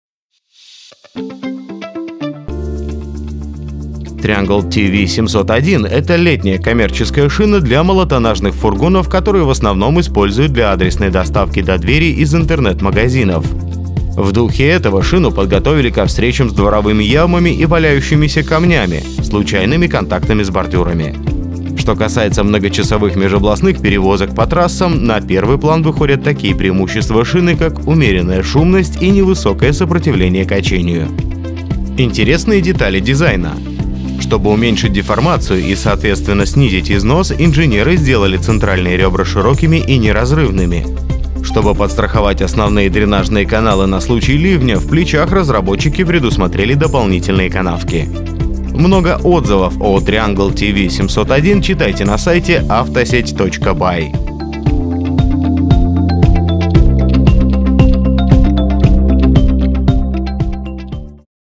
Triangle TV701 ☀️: обзор шины и отзывы ● Автосеть ●
Переходите по ссылке, чтобы узнать, в какой стране произвели Triangle TV701, какие оценки у этой шины в евроэтикетке, какая у неё начальная глубина канавок и сколько она весит.